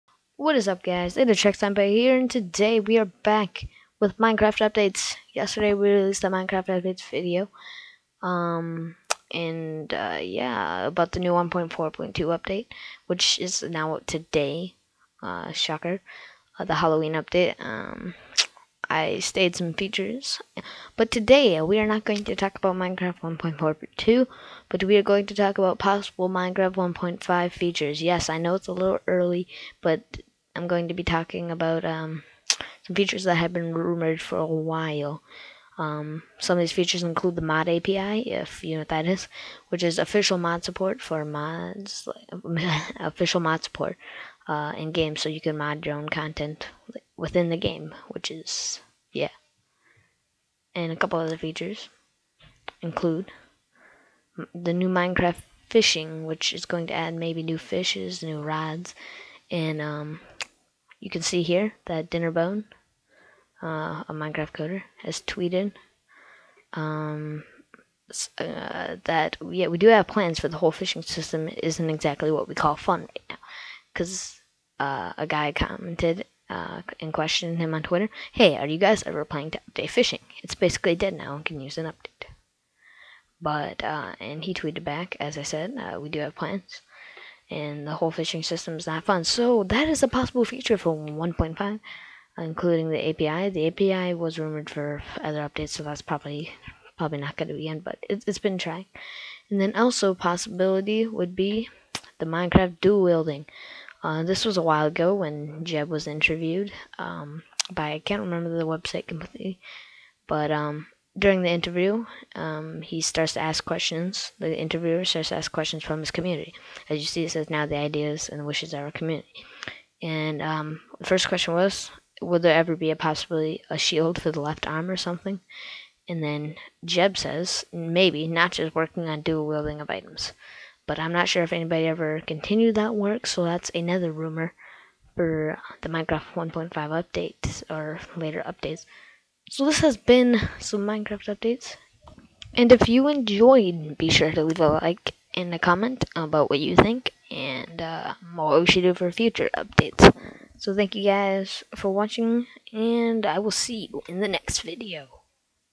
Minecraft 1.5 Features!! - NEW FISHING CONTENT! DUAL WIELDING, and Mod API - MC Updates #2 (Rumored)
I think this features would be GREAT! Tell me what you think?